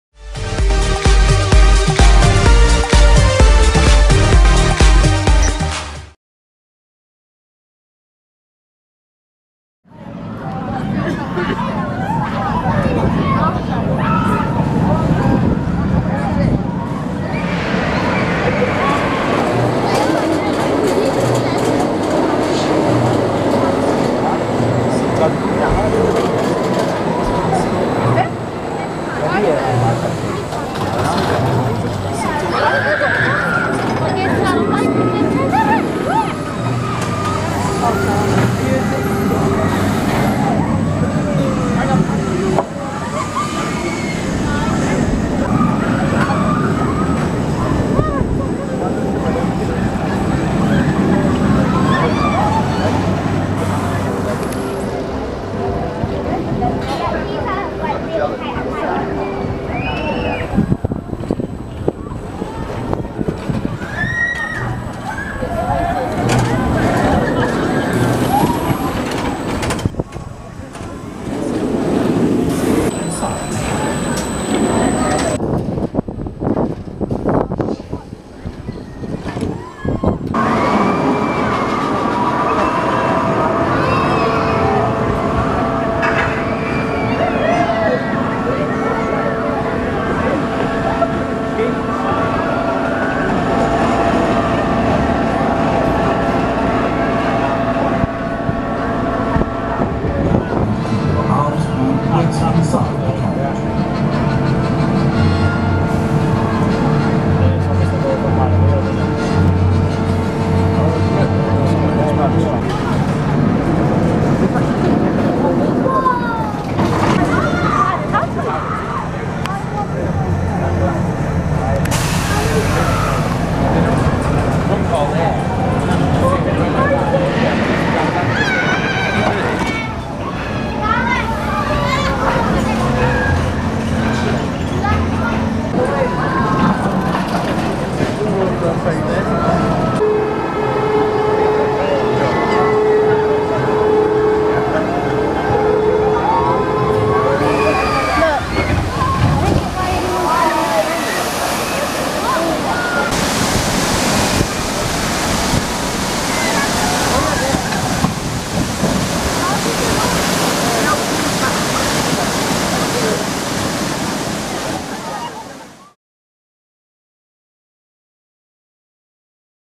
Flamingo Land - 11th August 2016 | 1080p HD
Footage of Flamingo Land, a theme park located in Malton, Yorkshire.  The park houses several unique rides including Velocity, Kumali, Mumbo Jumbo and Hero.

Download Auto Ride Count to automatically count the number of rides you go on!